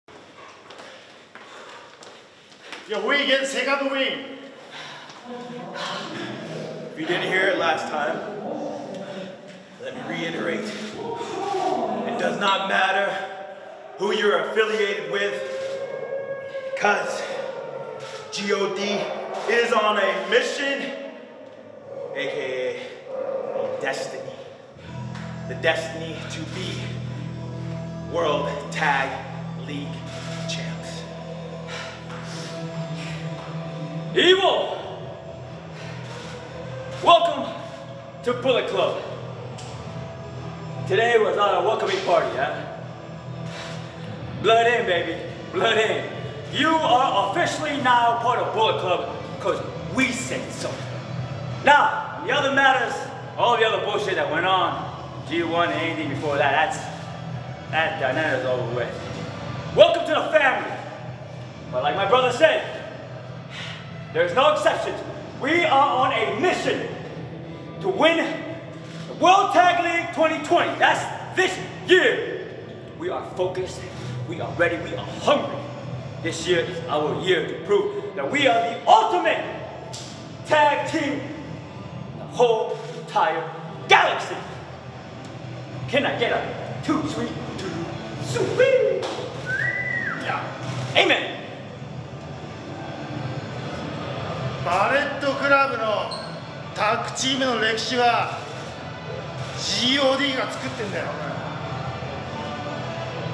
Guerrillas of Destiny victorious over BULLET CLUB competition! |World Tag League 2020
日本語字幕・English subtitles
World Tag League 2020 night 3, Korakuen Hall
Tama Tonga & Tanga Loa vs EVIL & Yujiro Takahashi